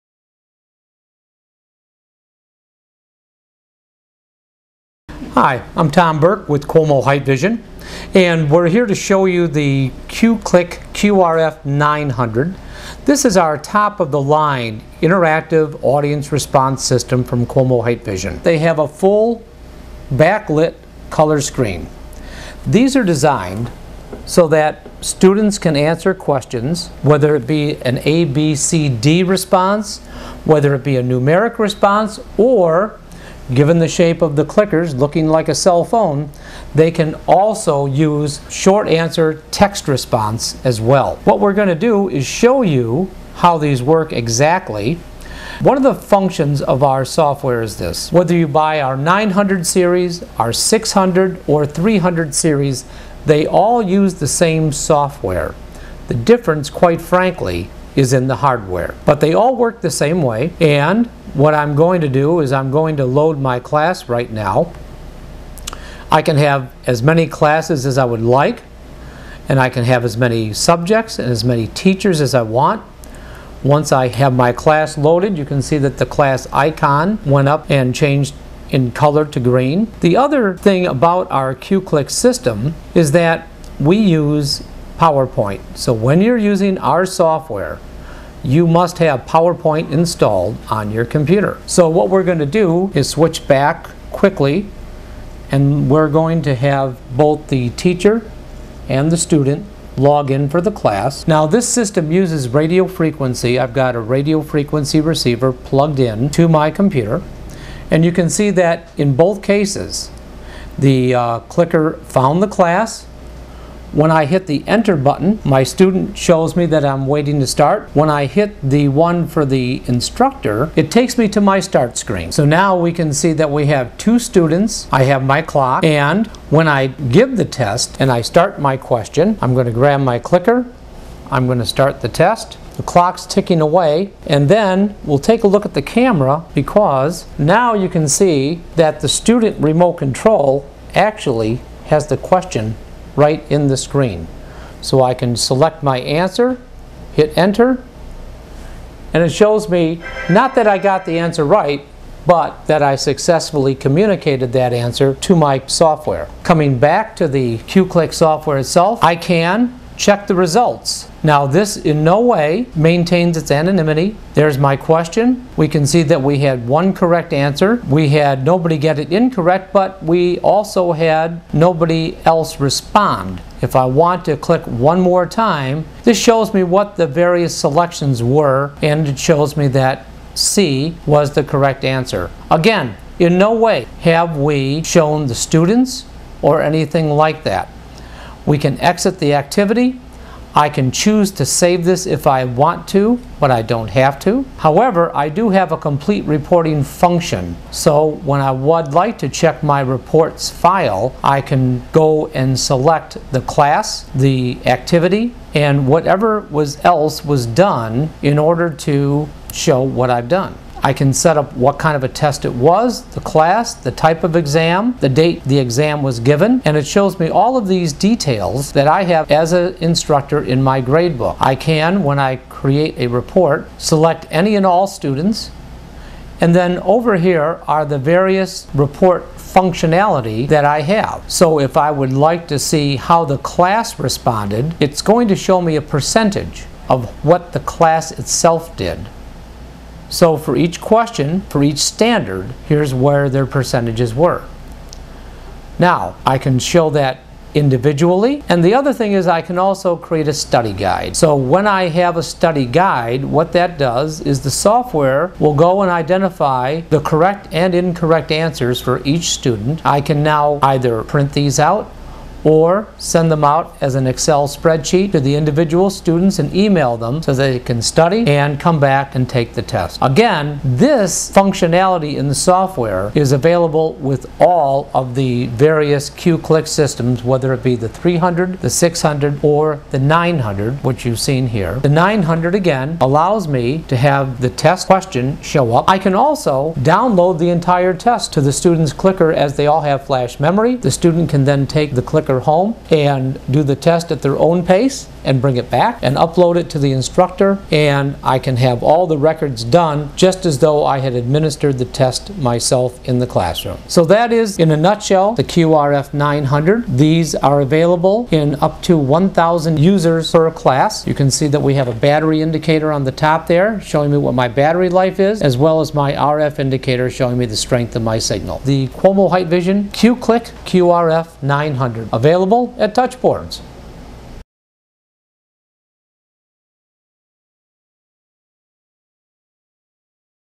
QOMO QRF900 Audience Response System
The QOMO QRF900 is an audience response system which has an interactive design that is sure to capture the attention of any audience. Compared with other QClick models, the QRF900 offers an enhanced user interface, making it even easier to operate for students and teachers alike. Even with its new larger color screen, battery life has been improved to lower the total cost of ownership. The wireless range of the remotes have been increased as well, to be sure this system will perform in the largest of lecture halls. The QRF900's sleek design is easy to handle and extremely functional. Its full color backlit display is pleasant to the eyes and makes on screen information easy to read. Instructors will find the new QRF900 easy to use and the built in laser pointer and wireless mouse will allow the instructors to engage their students with dynamic training and testing.

Video By Touchboards